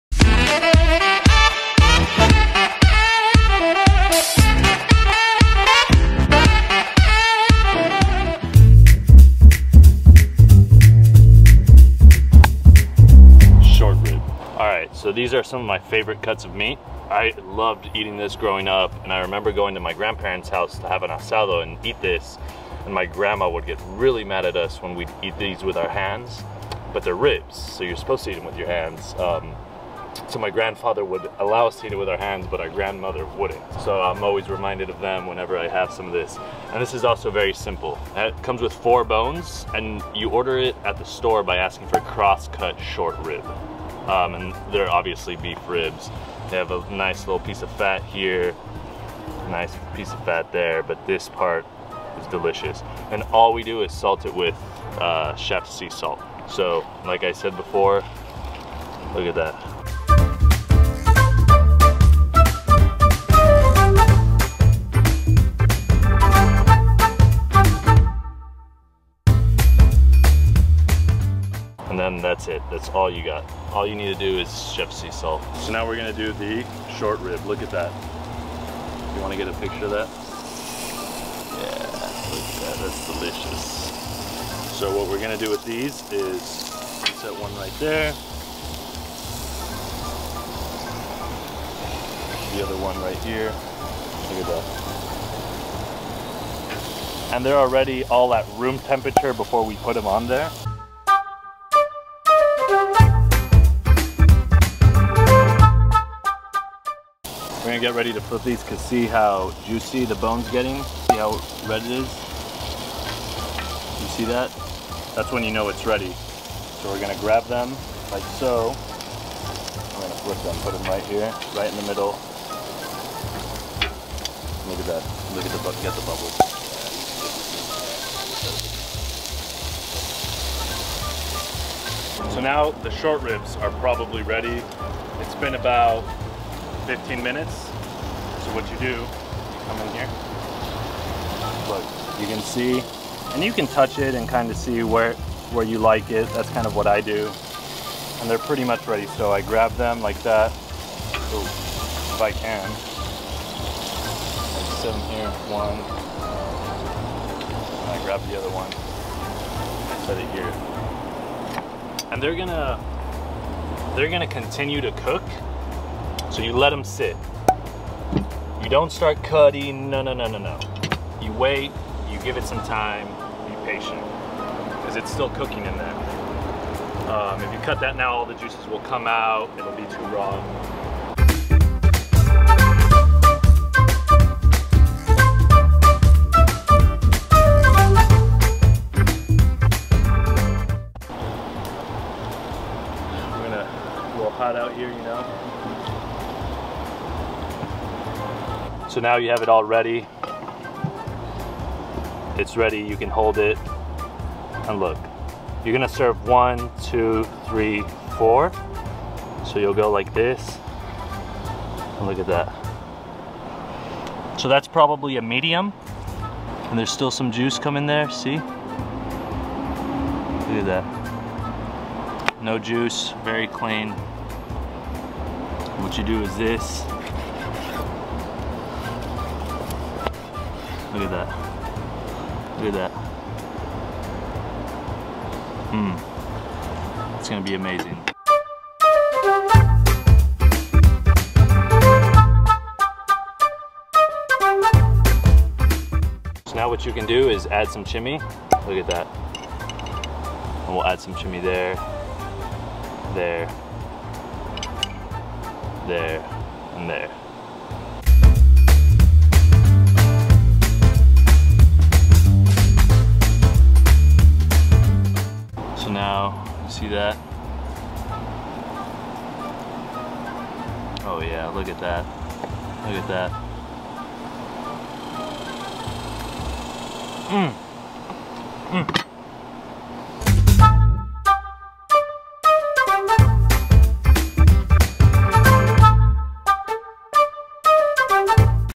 How To Grill Argentinian-style Cross-cut Short-ribs
Axel Brave, founder of AXEL Provisions, shows you how to cook the perfect Cross-cut Short rib, Argentinian-style.